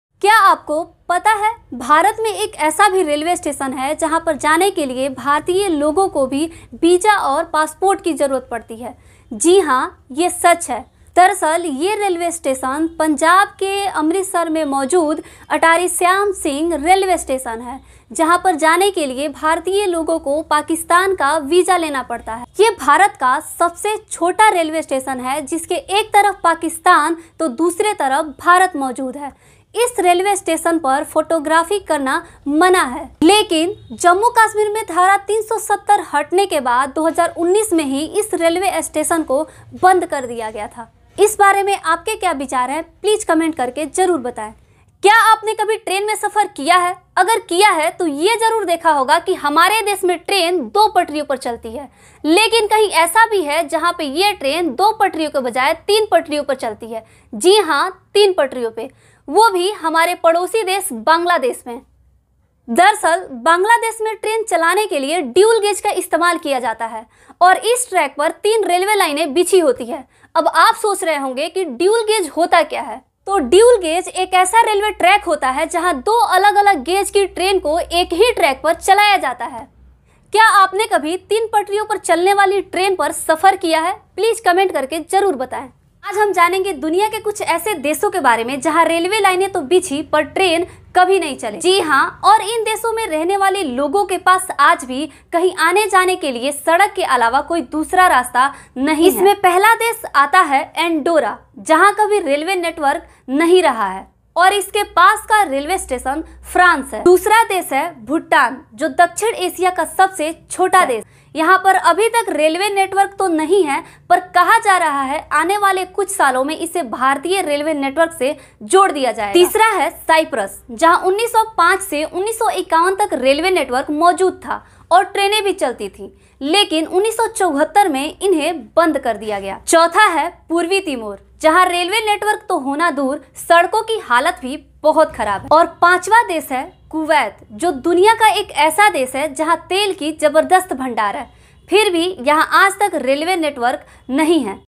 रेलवे स्टेशन पर जाने के लिए लगता हैं वीजा औऱ पासपोर्ट | ट्रेन से जुड़ी कुछ जरूरी बातें
I hope like this video.
So plz. like👍